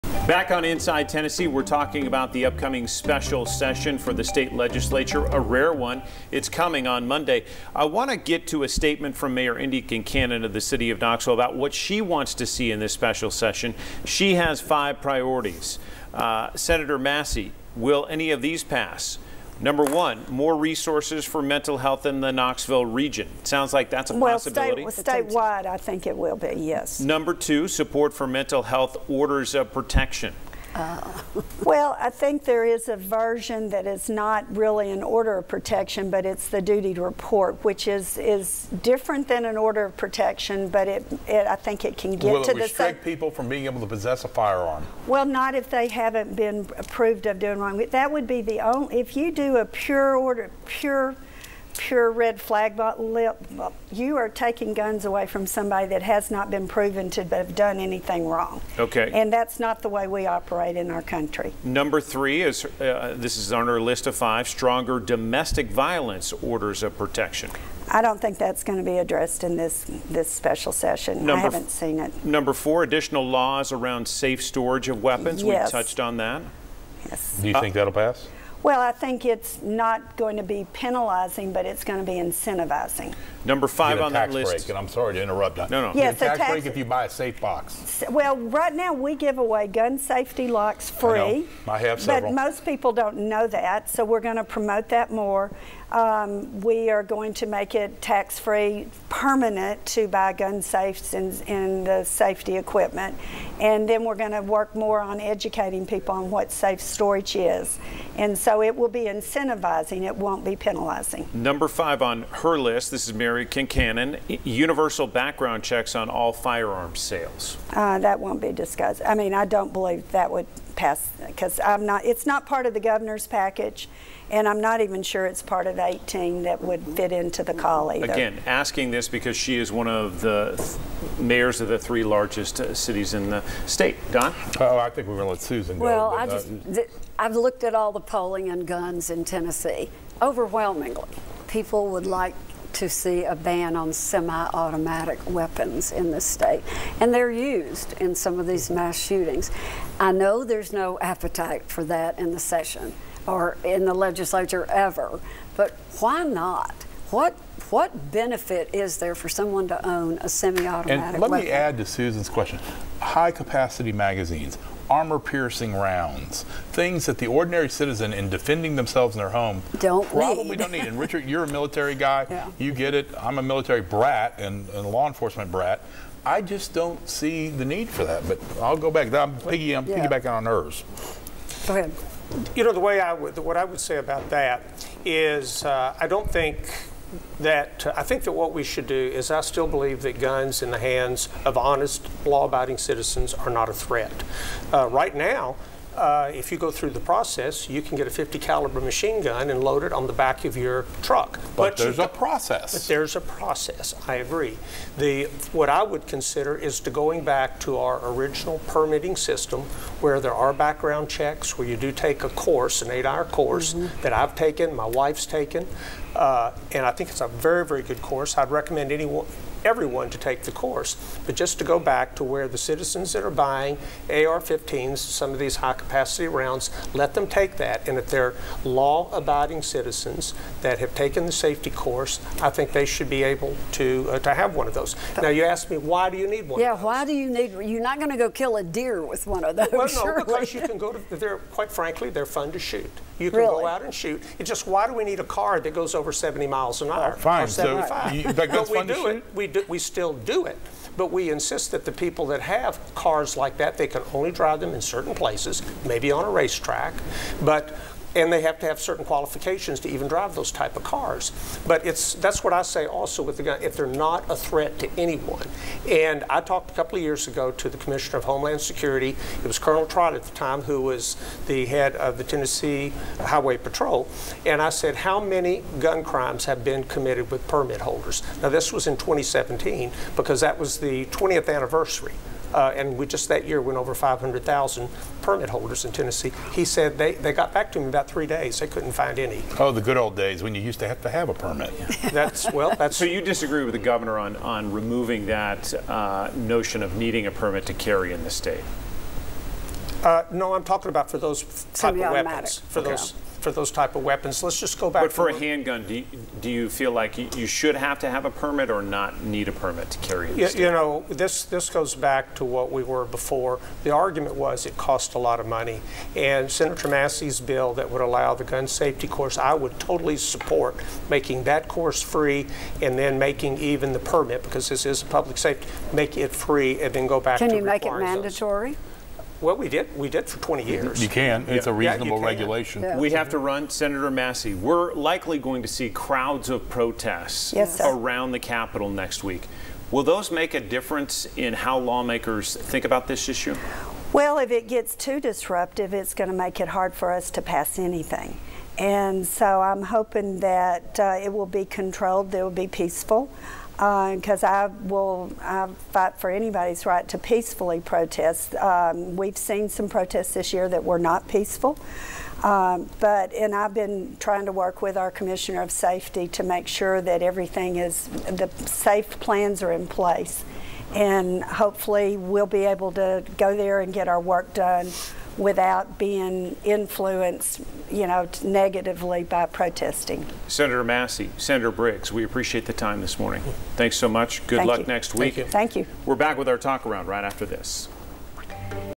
Inside TN: B. Massey, R. Briggs, Part 3
State Sens. Becky Duncan Massey and Richard Briggs talk about the special session.